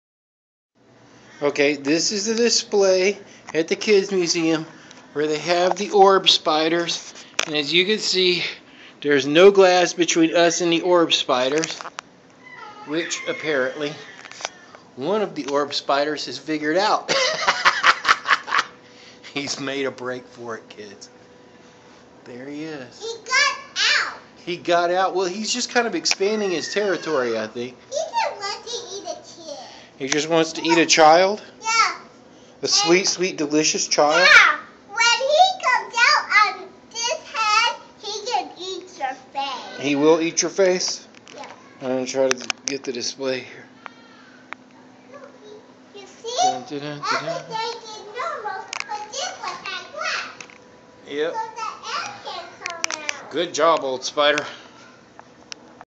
Orb Spider Breaks Out of Glassless Cage - Museum of Life and Science Durham NC
Look! The Orb Spider is expanding his territory. He's waiting for a kid to walk under his web so he can jump down and "eat your face" - as Nova puts it. There is not glass in this display, so the spiders are up to their own recognizance. It appears this one is a little more ambitious than the others...